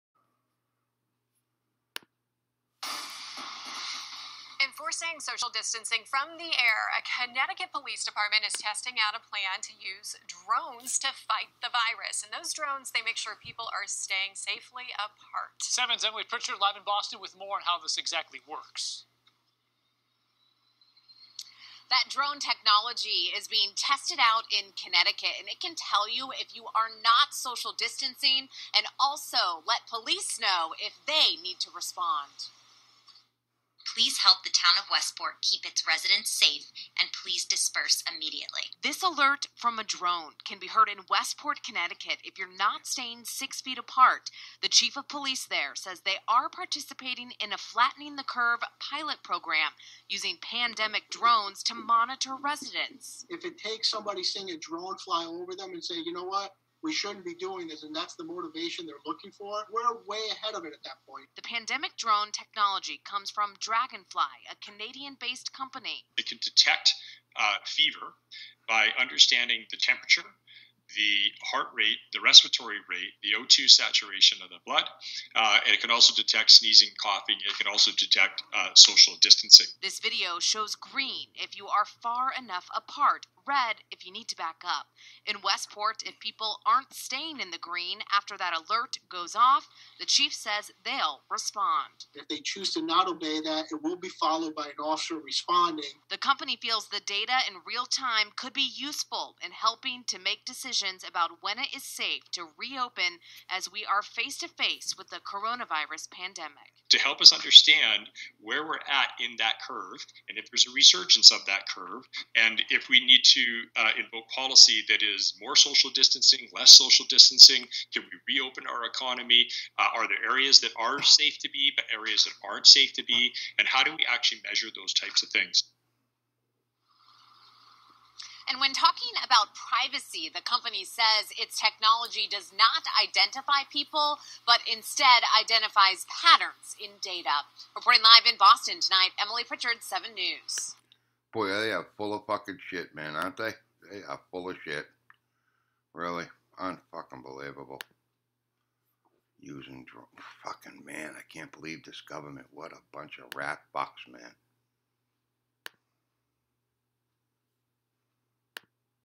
Using Drones For Social Distancing
The Bullshit just keeps piling higher and higher. More fear for the citizens, obey or the police will come.